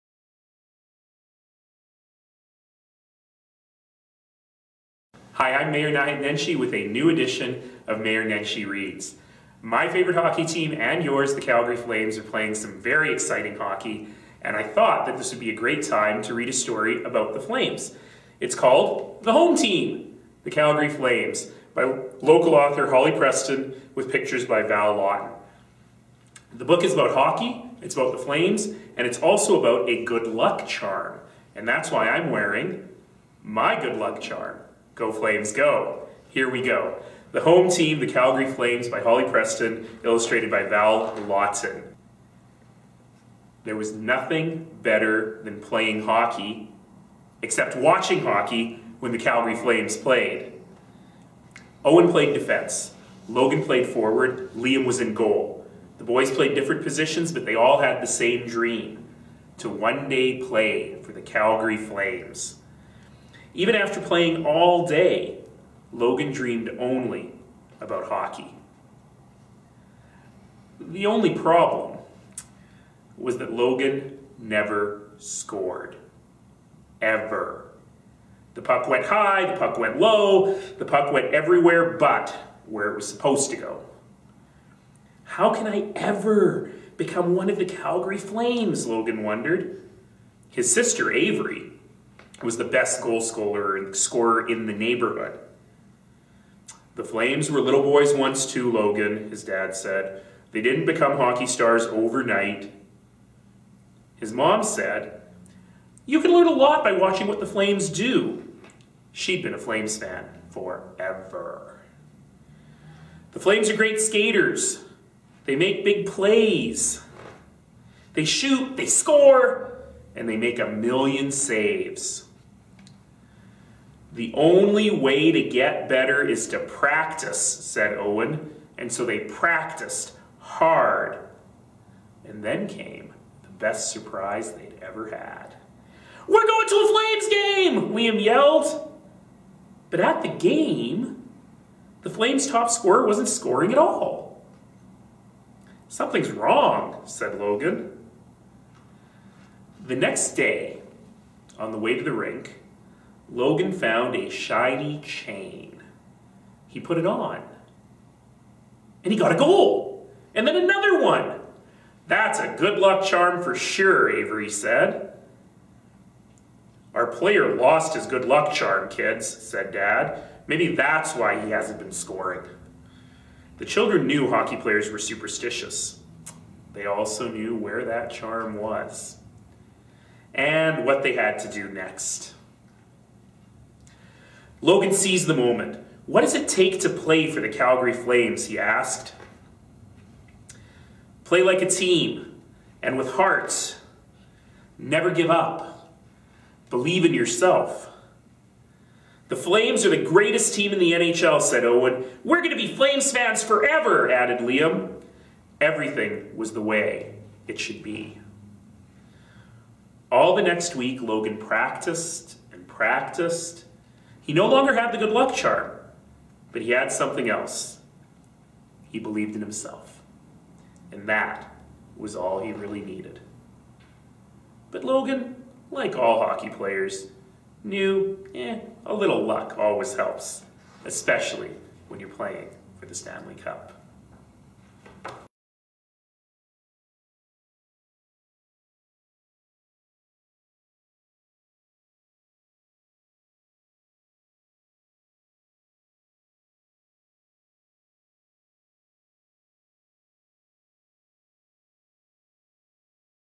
Mayor Nenshi Reads: The HOME Team Calgary Flames
GO FLAMES GO!

Mayor Naheed Nenshi reads "The HOME Team Calgary Flames" written and illustrated by Calgarians Holly Preston and Val Lawton.